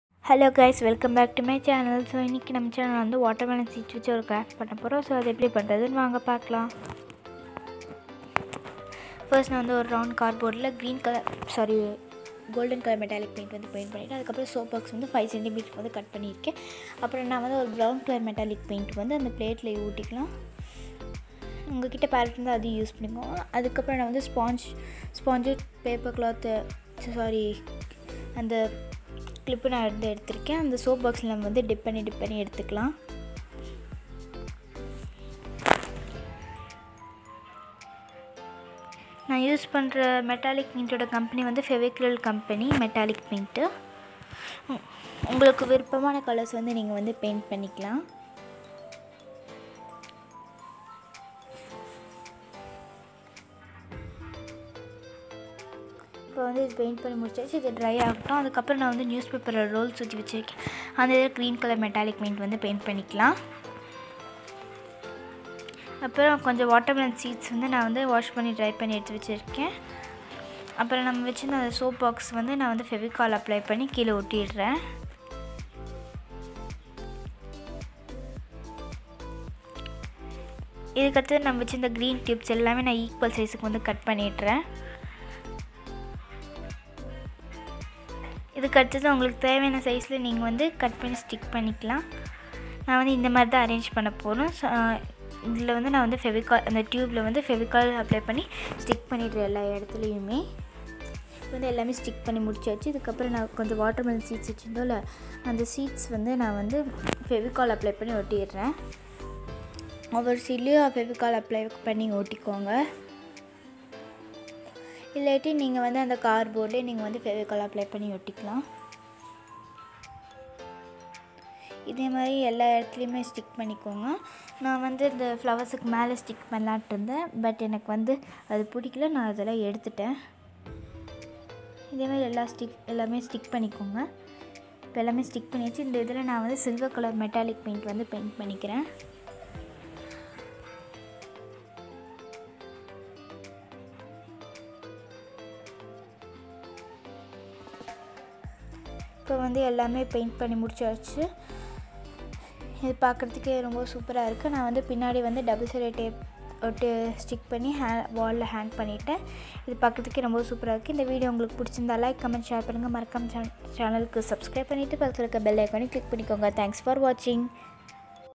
✂️wall decor with waste materials|cardboard, watermelon seeds, soapbox ✂️❣️| # shree's craftiti
✂️walldecor with waste materials|cardboard, watermelon seeds, soapbox ✂️|# shree's craftiti

     🔸🔸🔸Gift yourself creativity🔸🔸🔸